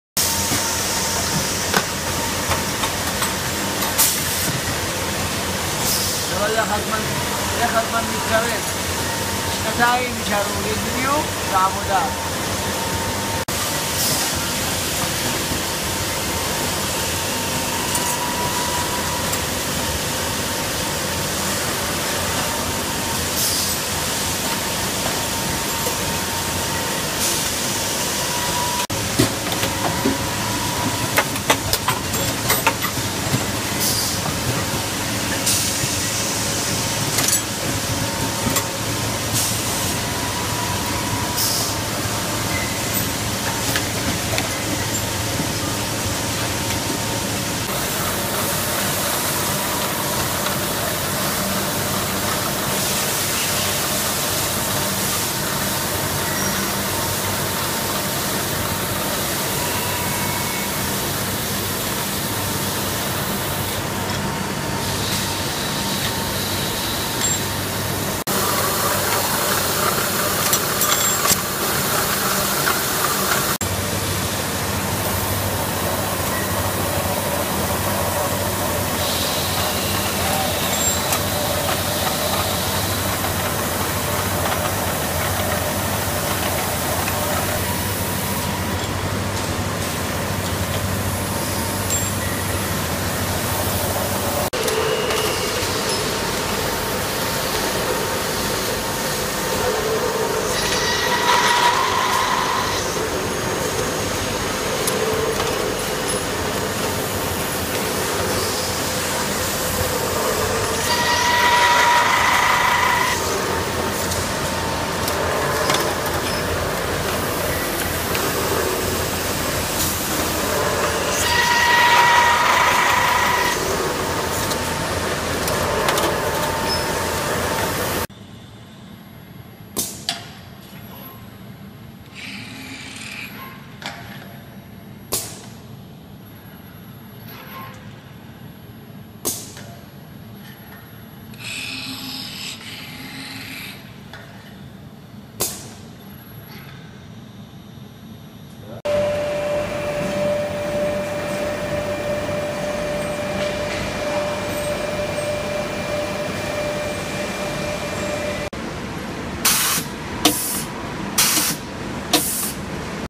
Natan's machine designs
My designs at work on the factory floor. I can't show the really cool ones because of customer confidentiality